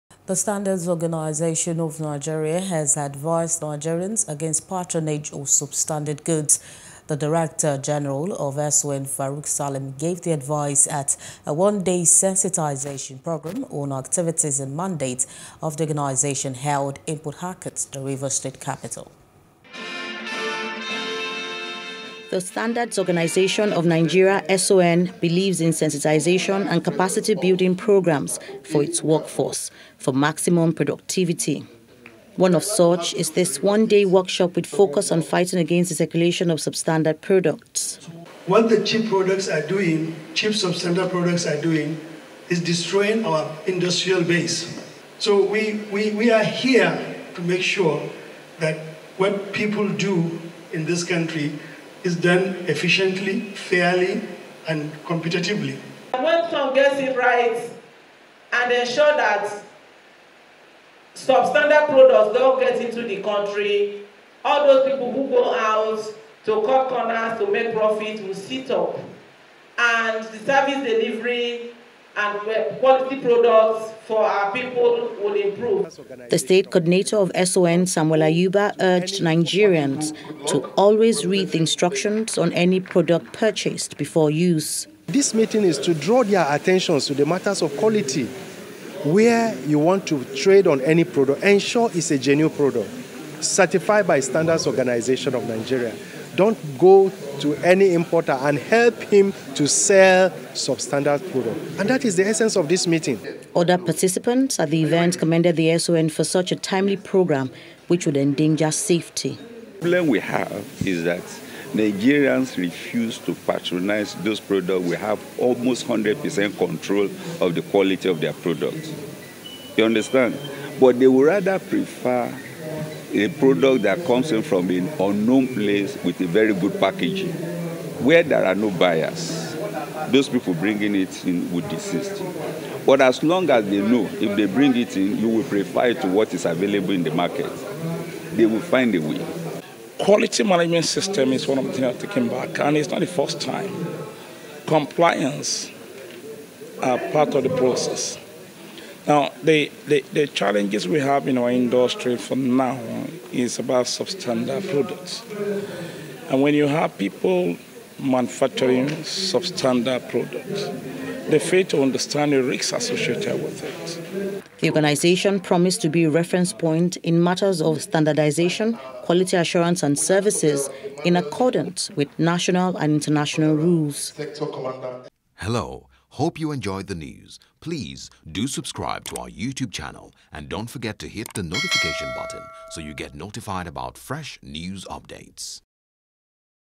Tackling Substandard Products: S.O.N. Caution Nigerians Against Purchasing Lesser Quality Products
The Standards Organisation of Nigeria (SON) has advised Nigerians against patronage of substandard goods.